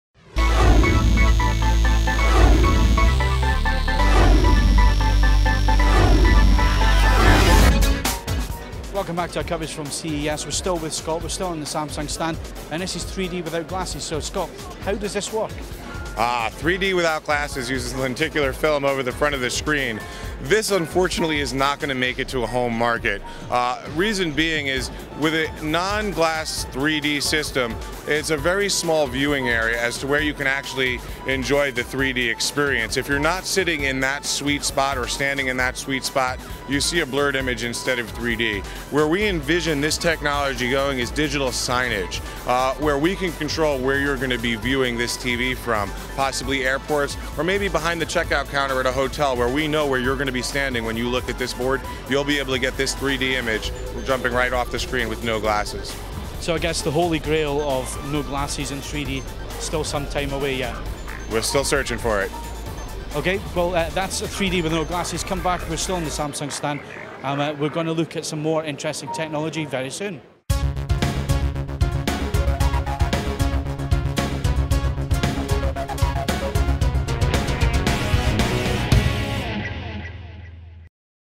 CES 2010 - Samsung 3D without the glasses
Scott from Samsung explains 3DTV without using glasses.  To comment on this video and the technology featured, go